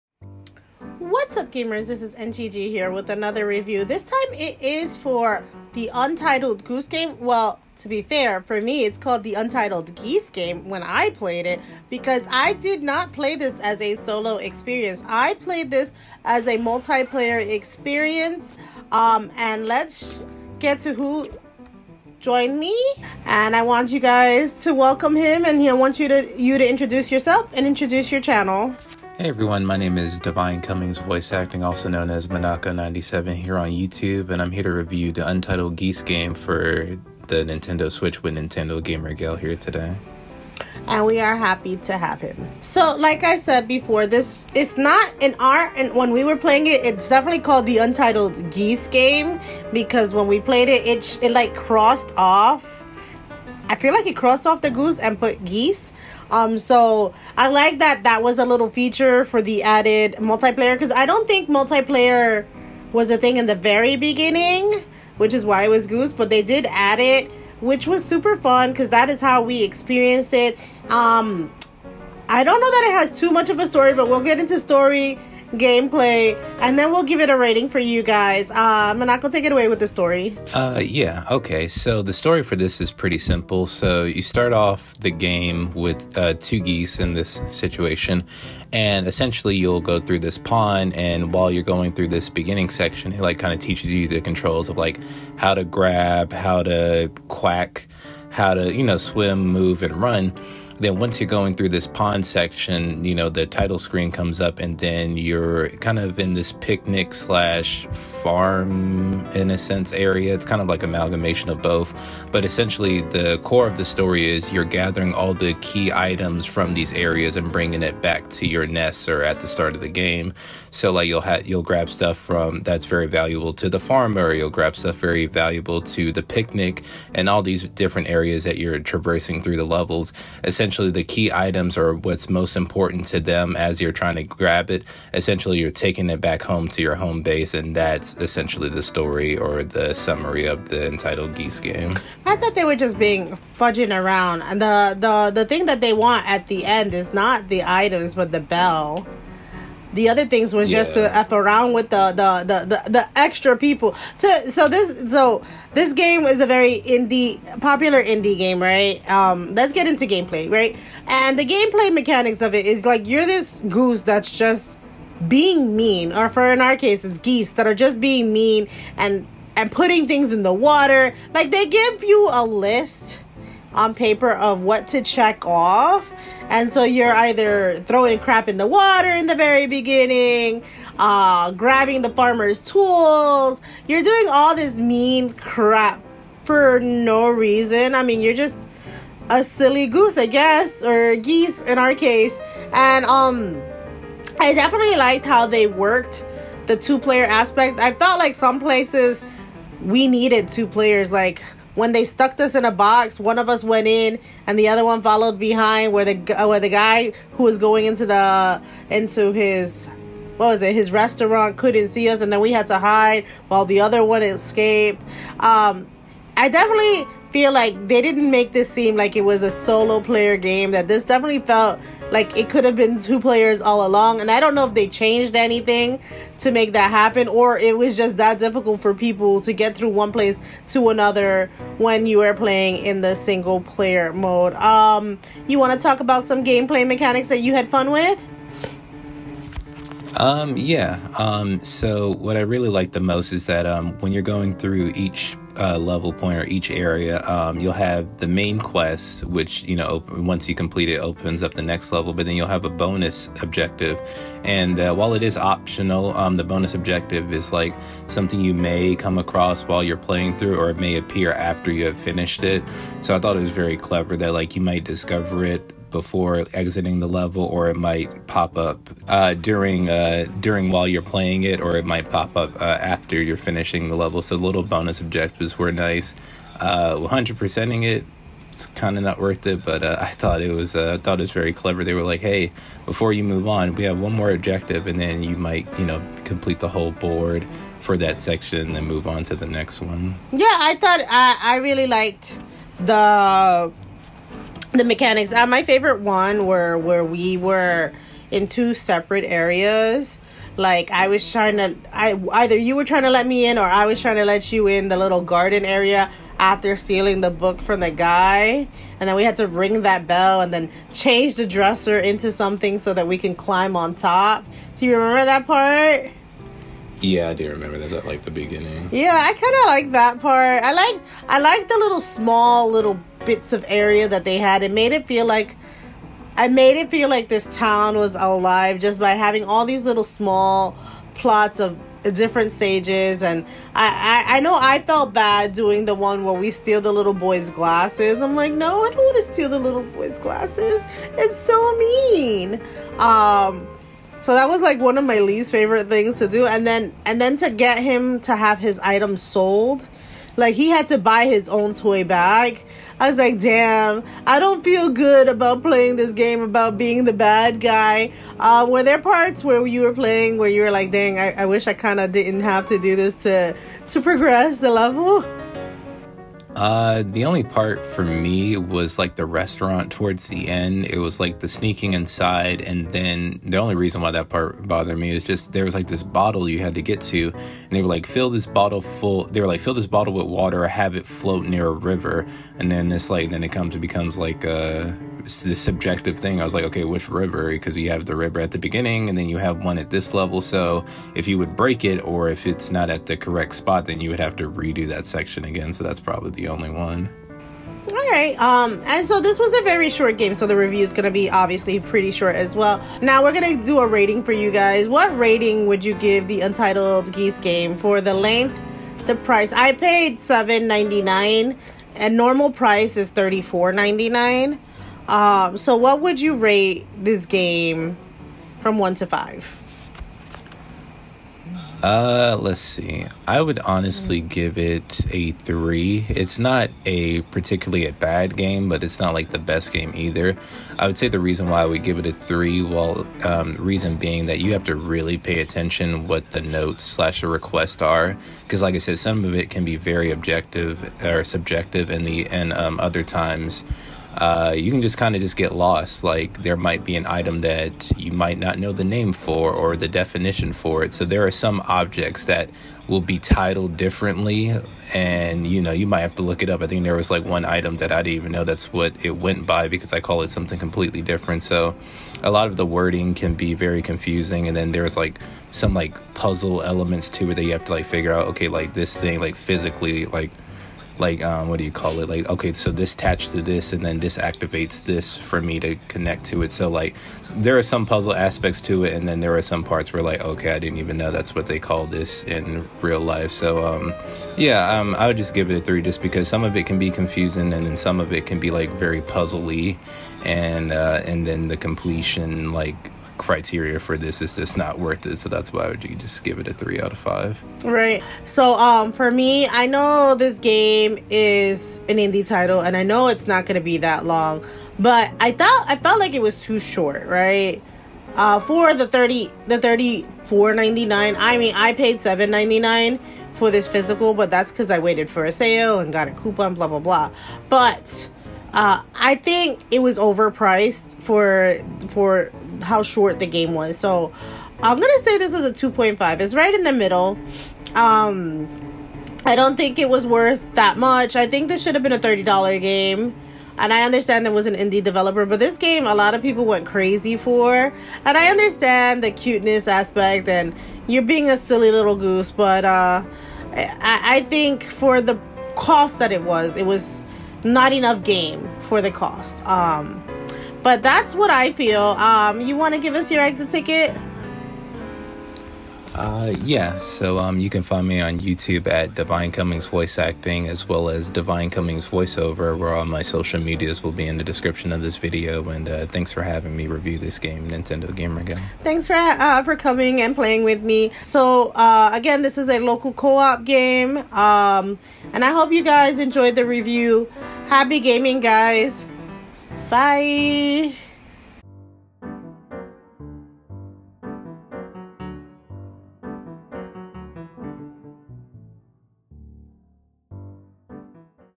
Untitled Geese Game a Beat Game Multiplayer Review for the Nintendo Switch
Whats up Gamers? I have another review for you this time its for Untitled Goose Game for the Nintendo Switch. We'll talk about Story, Gameplay & we'll rate it from 1-5. What rating do you think it will get? Watch and find out.  Happy Gaming.